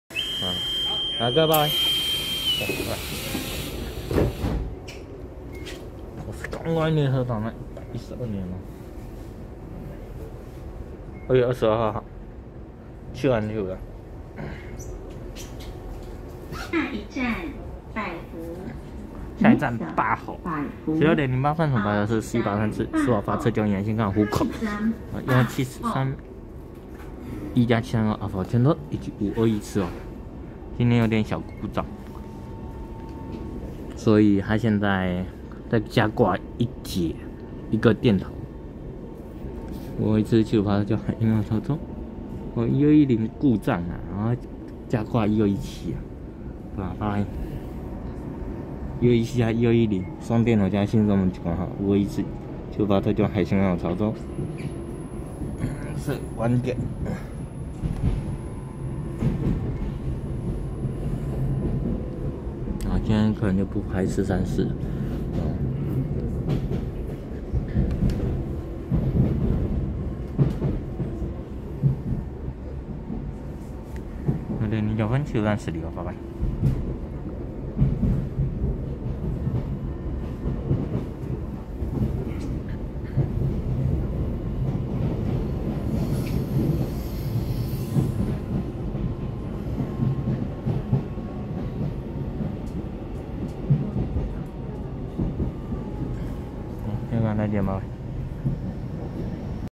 4183次蘇澳發車經由宜蘭線開往湖口的EMU731+EMU732阿福號區間車，和521次七堵發車經由海線開往潮州的E217救援+E210故障雙電頭+新式自動門莒光號
112/2/22七堵站16:08分拍攝4183次蘇澳發車經由宜蘭線開往湖口的EMU731+EMU732阿福號區間車，和521次七堵發車經由海線開往潮州的E217救援+E210故障雙電頭+新式自動門莒光號